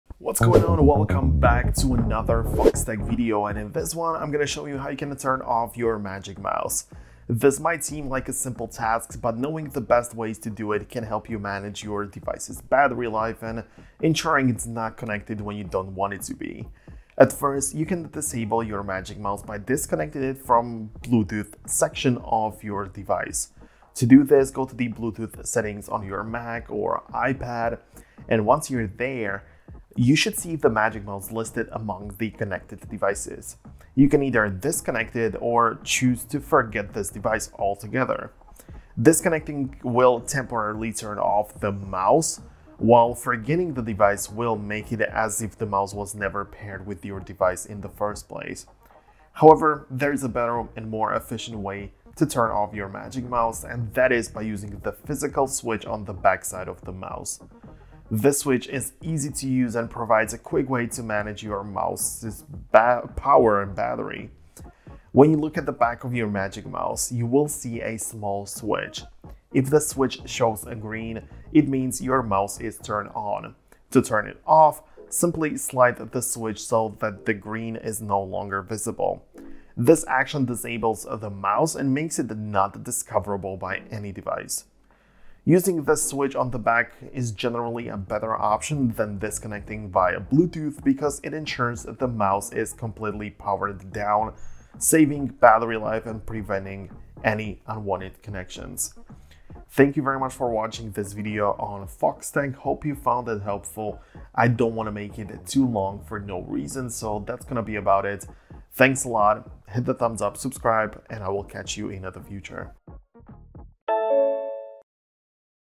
How to Turn Off Magic Mouse (tutorial)
In this video, we demonstrate how to turn off your Magic Mouse. Follow our simple instructions to power down your device properly, conserving battery life and ensuring your Magic Mouse is off when not in use. Watch now to learn the steps for turning off your Magic Mouse efficiently.